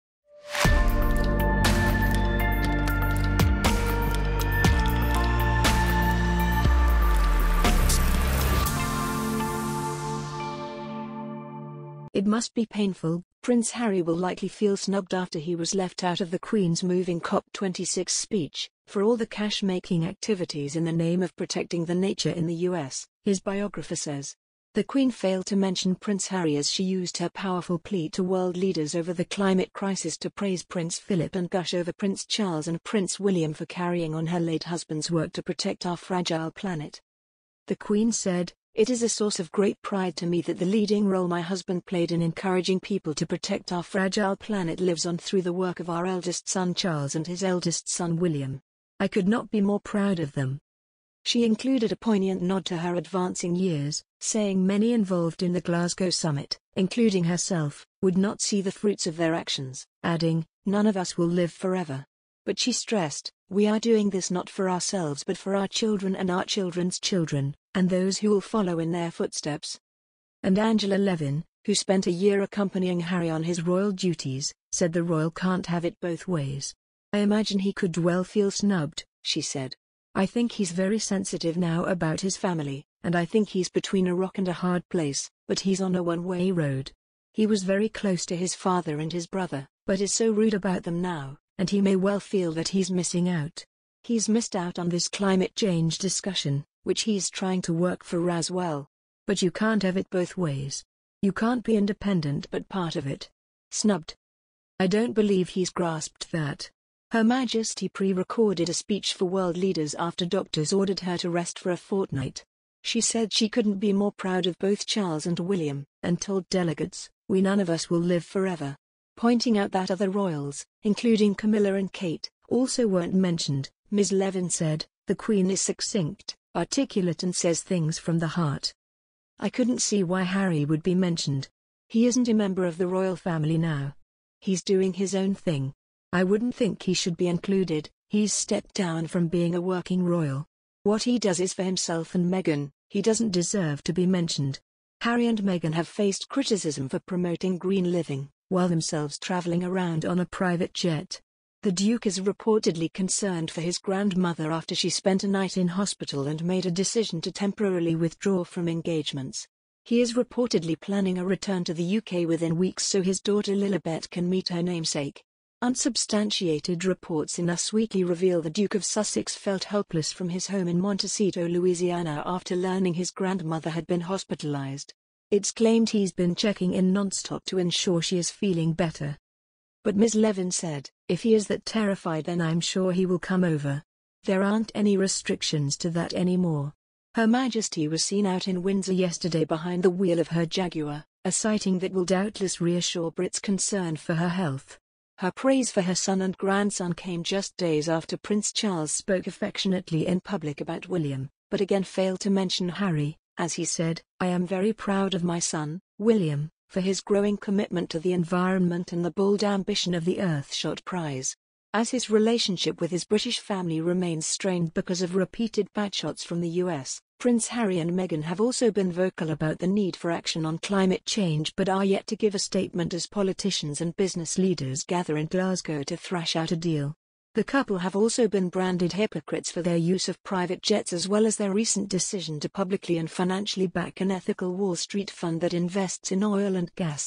PAIN! Harry ‘STABBED IN THE HEART’ After LEFT OUT In Queen Speech For Duke’s 'Eco-Activities' In US
PAIN! Harry ‘STABBED IN THE HEART’ After LEFT OUT In Queen Speech For Duke’s 'Eco-Activities' In The US